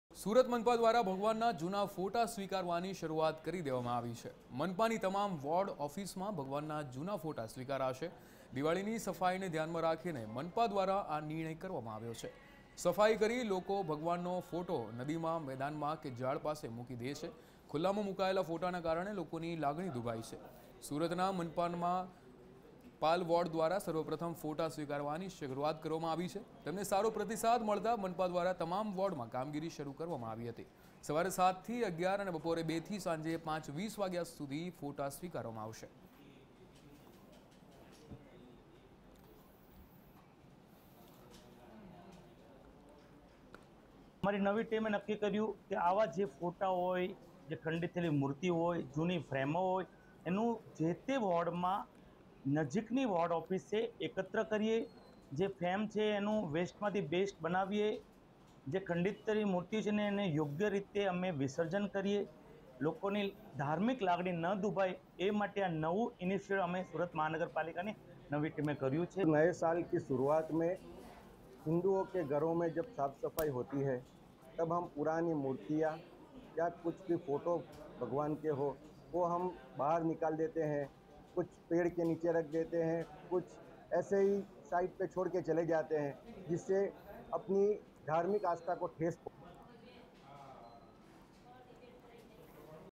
Surat: મનપા દ્વારા ભગવાનના જૂના ફોટા સ્વીકારવાની શરૂઆત | tv13 Gujarati
સુરત મનપા દ્વારા ભગવાનના જૂના ફોટા સ્વીકારવાની શરૂઆત કરવામાં આવી છે. મનપાની તમામ વોર્ડ ઓફિસમાં ભગવાનના જૂના ફોટા સ્વીકારાશે... દિવાળીની સફાઈને ધ્યાને રાખીને મનપા દ્વારા આ નિર્ણય કરવામાં આવ્યો છે... સફાઈ કરી લોકો ભગવાનના ફોટા નદીમાં, મેદાનમાં કે ઝાડ પાસે મૂકી દે છે. ખુલ્લામાં મુકાયેલા ફોટાને કારણે લોકોની લાગણી દુભાય છે. સુરતના મનપાના પાલ વોર્ડ દ્વારા સર્વપ્રથમ ફોટા સ્વીકારવાની શરૂઆત કરવામાં આવી છે. તેમને સારો પ્રતિસાદ મળતા મનપા દ્વારા તમામ વોર્ડમાં કામગીરી શરૂ કરવામાં આવી છે. સવારે 7થી 11 અને બપોરે 2થી સાંજે 5:20 વાગ્યા સુધી ફોટા સ્વીકારવામાં આવશે...